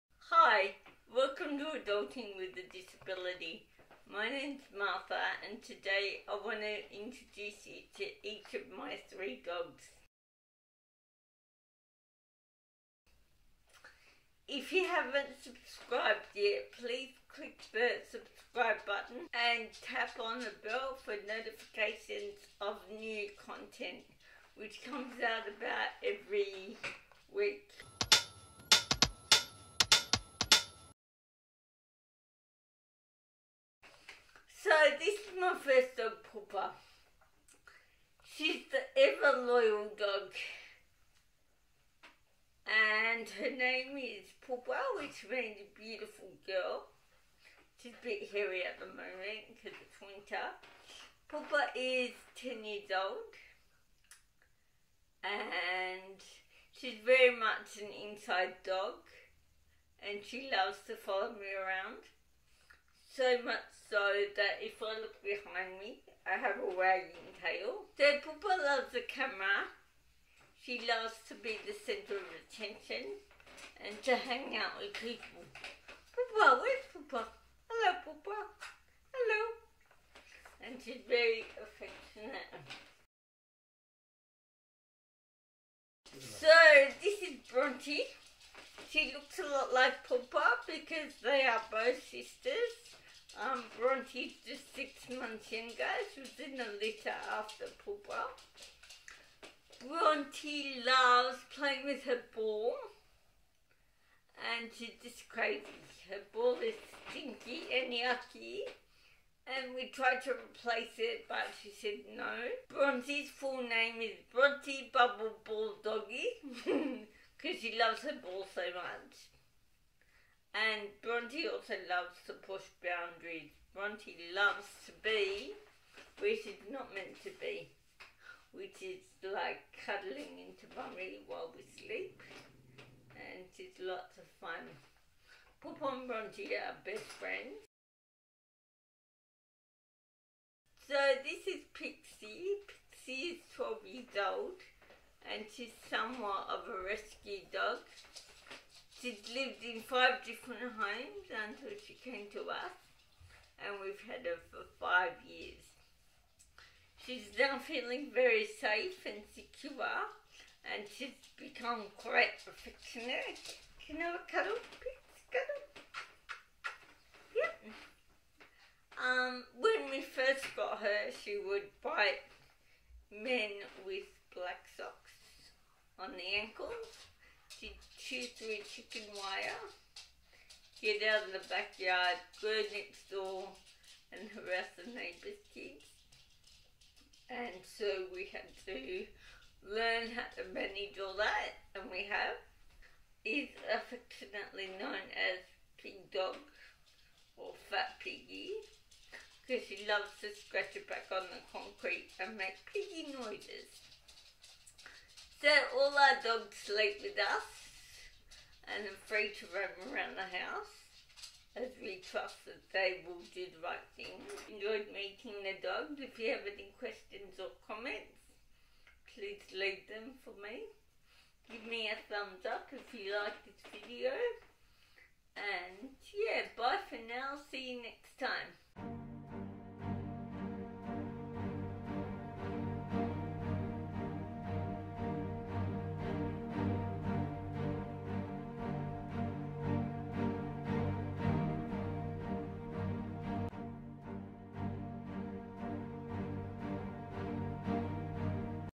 Introducing Pupa, Bronte & Pixie
Hi, welcome to Adulting With a Disability, my name is Martha, today I want to introduce each of my dogs to you and share their likes, dislikes and funny mannerisms.

My first dog is the ever loyal Pupa and is 10 years old.  Pupa means ‘Beautiful girl’ in Maltese.  Pupa is very much an inside dog and she loves to follow me around.  Her favourite place to walk is behind me, under my seat, so much so that when I look behind me I have a wagging tail!

Bronte is the second dog we got.  She is a full sister to Pupa – same mum and dad but the next litter.  Bronte is 9 ½ and is ball crazy.  Just one stinky, yucky ball we have tried to replace but she won’t let us.  Her full name is Bronte, Bubble Ball doggie!  Bronte loves to push boundaries as to where I want her to sleep and most of the time she gets away with it.

Then we have Pixie who is 12.  She is somewhat of a rescue dog having lived in 5 previous homes.  We have had her for 5 years.
She is now feeling very safe and behaves quite affectionately but it hasn’t always been that way.  She used to bite men’s ankles and tear into food, tissues and chicken wire.  She used to always escape from the yard too.  Pixie is affectionately known as ‘Fat piggy’ as she scratches her back and makes pig noises.

All dogs sleep with us, are free to roam the house and are trusted..

‘These views and opinions are my own'